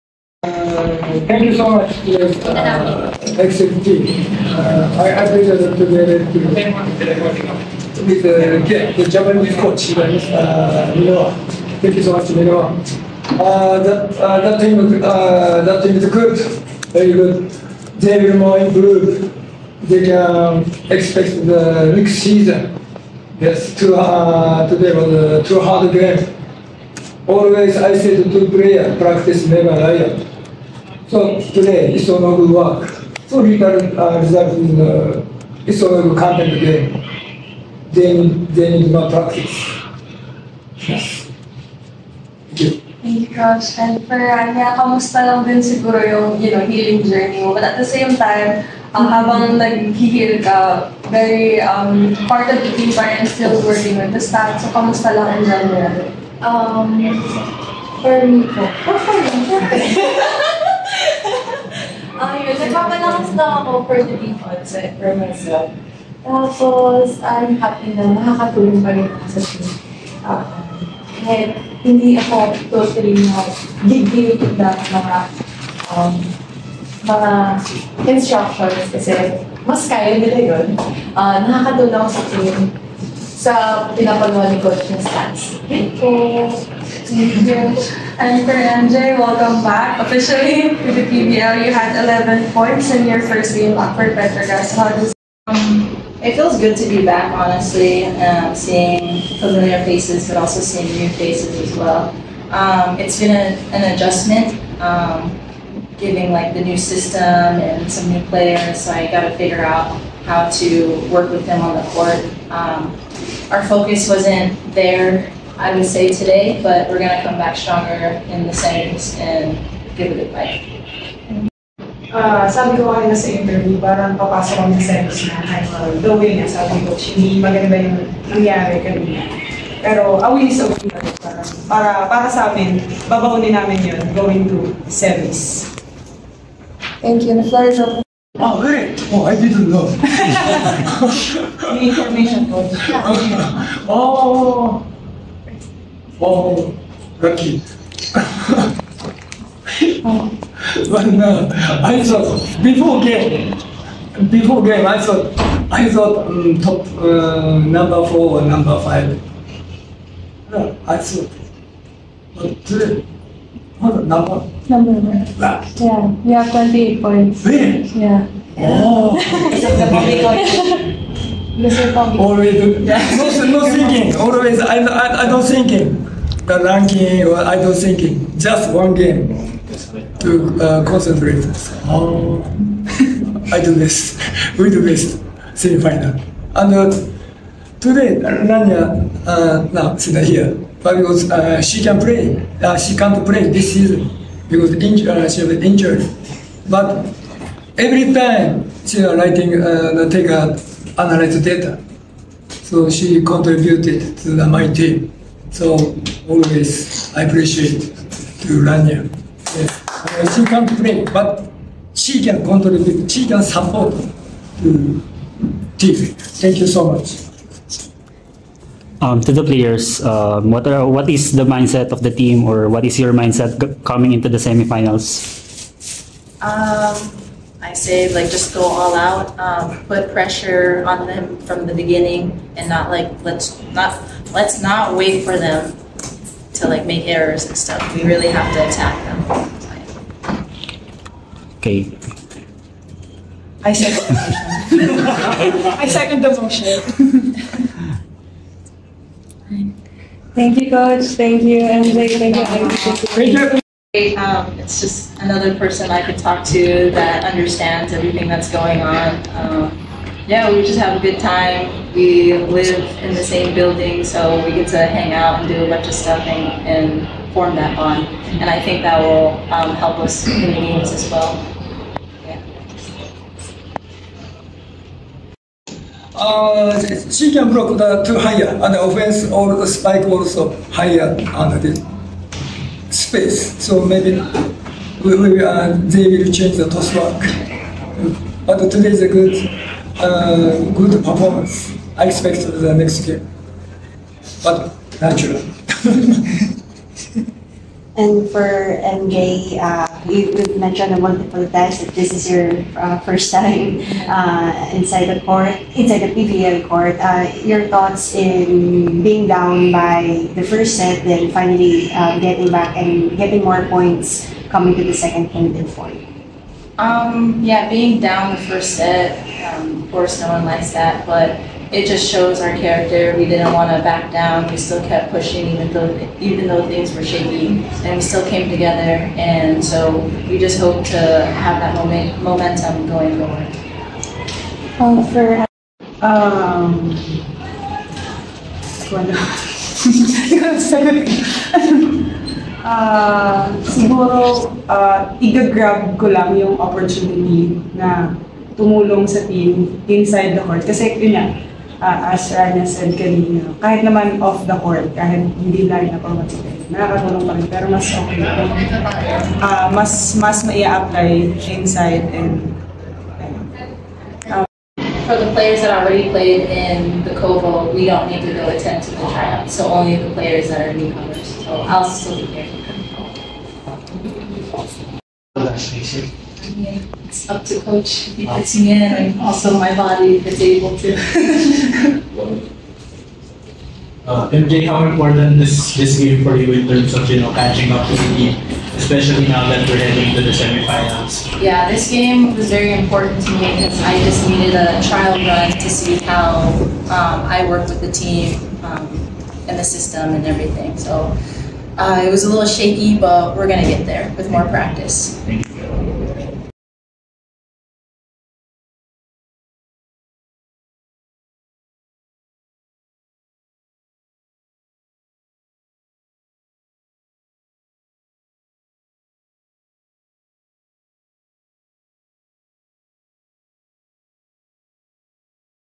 PVL: Petro Gazz clinches top seed in 2024 All-Filipino | Soundbites
Petro Gazz claimed the top seed in the 2024 Premier Volleyball League (PVL) All-Filipino Conference at the expense of Nxled, 22-25, 25-23, 25-23, 25-22 on Saturday at the PhilSports Arena in Pasig City.